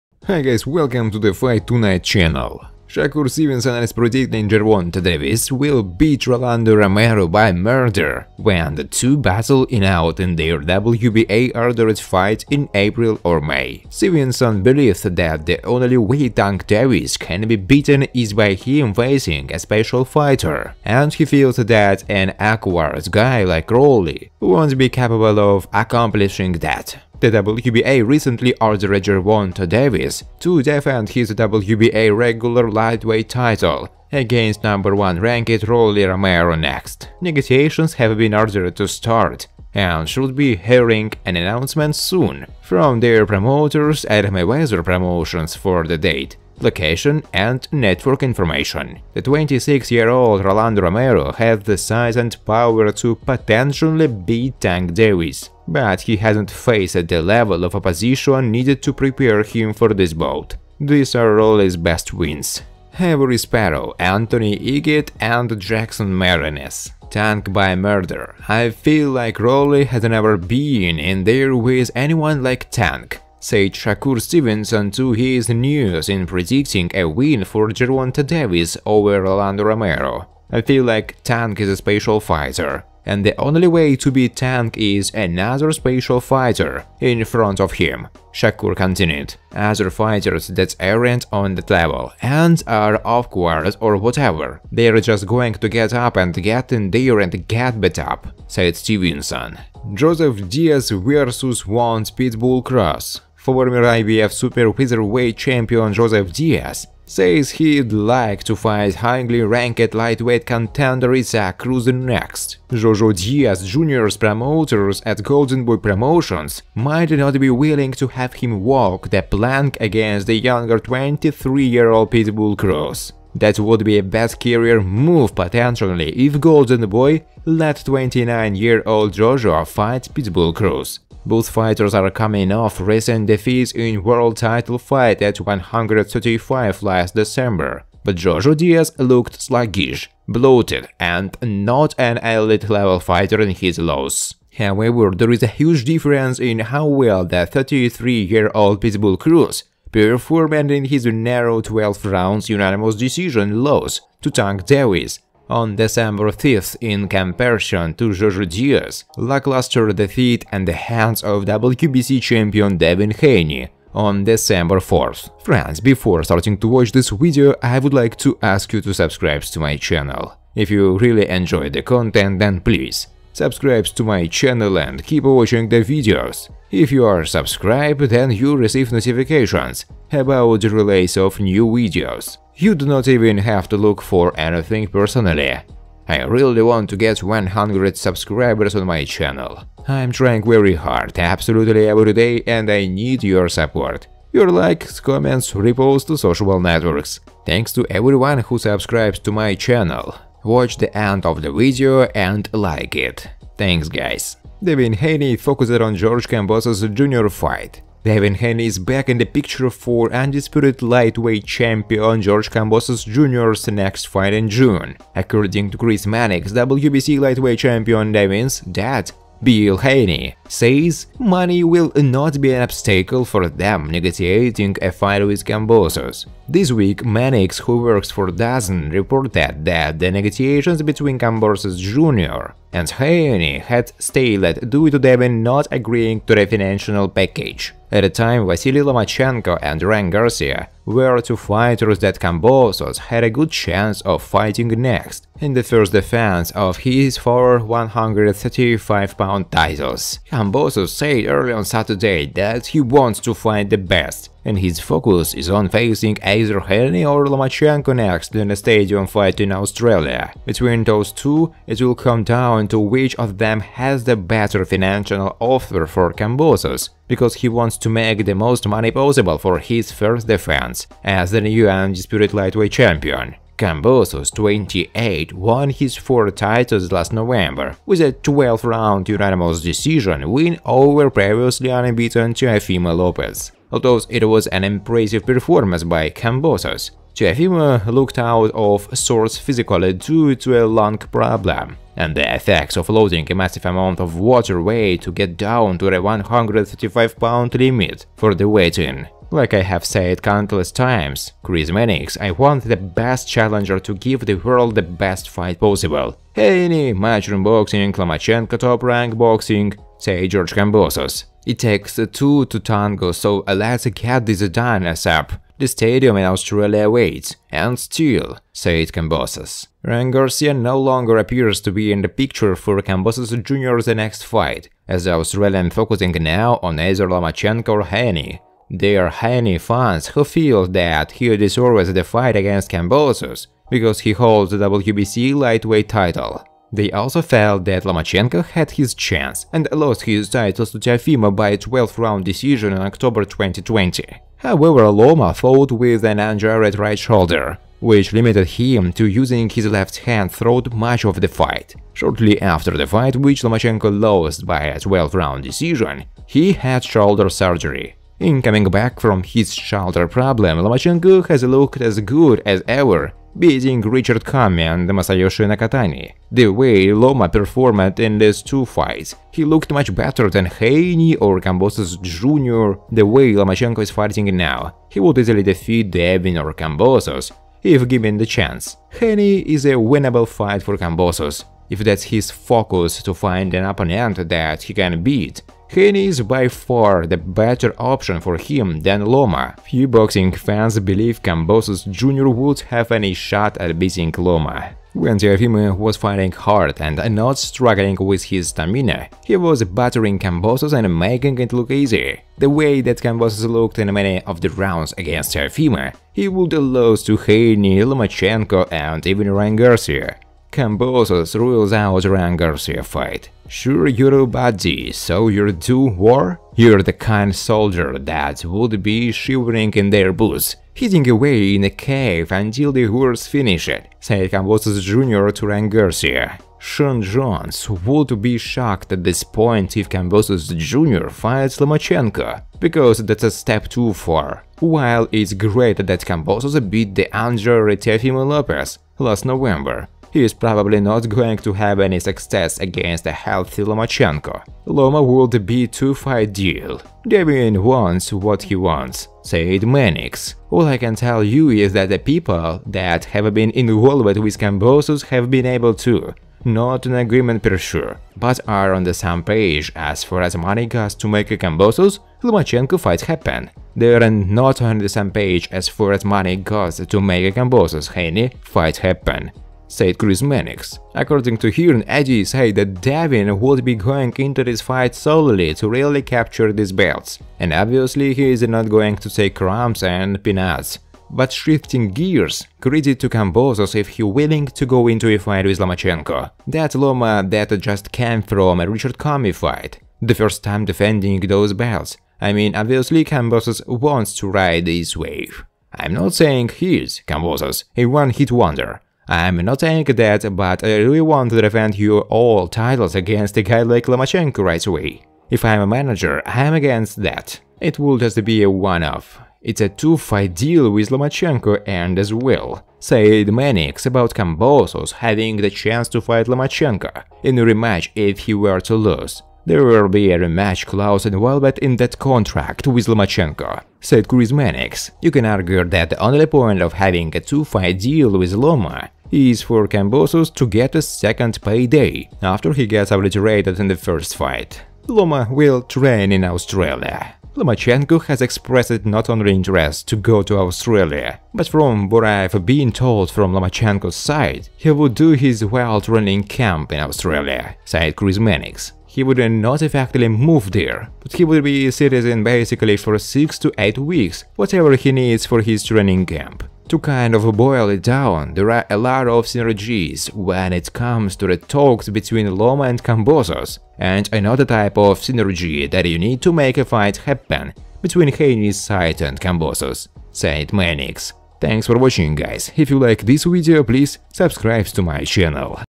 EPIC! "GERVONTA DAVIS WILL BEAT ROLANDO ROMERO" - Shakur Stevenson / KAMBOSOS TURNS DOWN RYAN GARCIA
Shakur Stevenson is predicting Gervonta ‘Tank’ Davis will beat #1 WBA Rolando ‘Rolly’ Romero by “Murder’  when the two battle it out in their WBA-ordered fight in April or May.

Stevenson believes that the only way Tank Davis can be beaten is by him facing a “special fighter,” and he feels that an “awkward” guy like Rolly won’t be capable of accomplishing that.

The World Boxing Association recently ordered Gervonta (26-0, 24 KOs) to defend his WBA ‘regular’ lightweight title against #1 ranked Rolly Romero (14-0, 12 KOs) next.

Negotiations have been ordered to start and should be hearing an announcement soon from their promoters at Mayweather Promotions for the date, location, and network information.

The 26-year-old Rolly Romero has the size and power to potentially beat Tank Davis, but he hasn’t faced the level of opposition needed to prepare him for this bout. These are Rolly’s best wins:

“Tank by murder. I feel like Rolly has never been in there with anyone like Tank,” said Shakur Stevenson to ESNEWS in predicting a win for Gervonta Davis over Rolando Romero.

“I feel like Tank is a special fighter, and the only way to beat Tank is another special fighter in front of him,” Shakur continued on WHY Gervonta beats Rolly Romero.

“Other fighters that aren’t on that level and are awkward or whatever, they’re just going to get up and get in there and get beat up,” said Stevenson.

Joseph ‘Jojo’ Diaz Jr. Wants Pitbull Cruz
Former IBF super featherweight champion Joseph  ‘Jojo’ Diaz Jr. says he’d like to fight highly ranked lightweight contender Isaac ‘Pitbull Cruz next.

‘Jojo’ Diaz Jr’s promoters at Golden Boy Promotions might not be willing to have him walk the plank against the younger 23-year-old Pitbull Cruz. That would be a bad career move potentially if Golden Boy let 29-year-old Jojo fight Pitbull Cruz.

Both fighters are coming off recent defeats in world title fights at 135 last December,  but Jojo Diaz looked sluggish, bloated, and not an elite-level fighter in his loss.

However, there’s a huge difference in how well the 23-year-old Pitbull Cruz (22-2-1, 15 KOs) performed in his narrow 12 round unanimous decision loss to Tank Davis on December 5th in comparison to Jojo Diaz’s lackluster defeat at the hands of WBC champion Devin Haney on December 4th.

Devin Haney Focused On George Kambosos Jr. Fight

Devin Haney is back in the picture for undisputed lightweight champion George Kambosos Jr’s next fight in June. According to Chris Mannix, WBC lightweight champion Devin’s dad, Bill Haney, says “Money will NOT be an obstacle” for them negotiating a fight with Kambosos.

This week, Mannix, who works for DAZN, reported that the negotiations between Kambosos Jr. and Haney (27-0, 15 KOs) had “stalled” due to Devin not agreeing to the financial package.

At the time, Vasily Lomachenko and Ryan Garcia were to fighters that Kambosos had a good chance of fighting next in the first defense of his four 135-lb titles.

Kambosos said earlier on Saturday that he wants to fight the best, and his focus is on facing either Haney or Lomachenko next in a stadium fight in Australia.

Between those two, it’ll come down to which of them has the better financial offer for Kambosos (20-0, 10 KOs) because he wants to make the most money possible for his first defense as the new undisputed lightweight champion.

Kambosos, 28, won his four titles last November with a 12 round unanimous decision win over previously unbeaten Teofimo Lopez.

Although it was an impressive performance by Kamaboso, Teofimo looked out of sorts physically due to a lung problem, and the effects of losing a massive amount of water weight to get down to the 135-lb limit for the weigh-in.

“Like I have said countless times, Chris Mannix, I want the best challenger to give the world the best fights possible. Haney, Matchroom boxing, Lomachenko Top Rank boxing,” said @GeorgeKambosos.

It takes two to tango, so let’s get this done ASAP. The stadium in Australia awaits. And still!” said Kambosos.

Ryan Garcia no longer appears to be in the picture for Kambosos Jr’s next fight, as the Australian focusing now on either Lomachenko or Haney.

There are Haney fans who feel that he deserves the fight against Kambosos because he holds the WBC lightweight title. They also felt that Lomachenko had his chance, and lost his titles to Teofimo by a 12 round decision in October 2020.

However, Lomachenko fought with an injured right shoulder, which limited him to using his left hand through much of the fight. Shortly after the fight, which Lomachenko lost by a 12 round decision, he had shoulder surgery.

In coming back from his shoulder problem, Lomachenko has looked as good as ever, beating Richard Commey and Masayoshi Nakatani. The way Lomachenko performed in those two fights, he looked much better than Haney or Kambosos Jr.